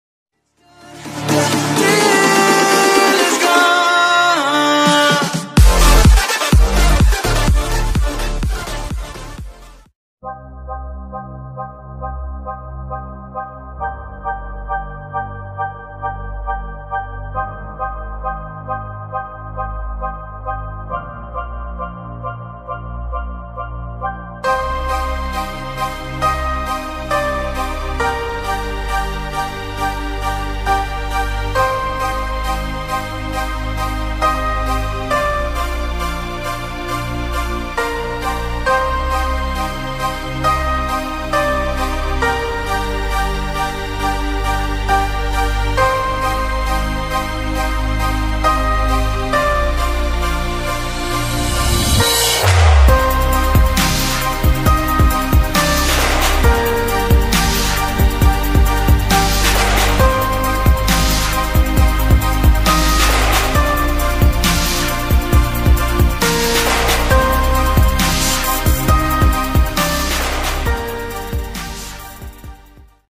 What is the Full form of mobile // ম’বাইলৰ full form জানি লওক // general knowledge //
Do you know mobile full form. Lets play this video.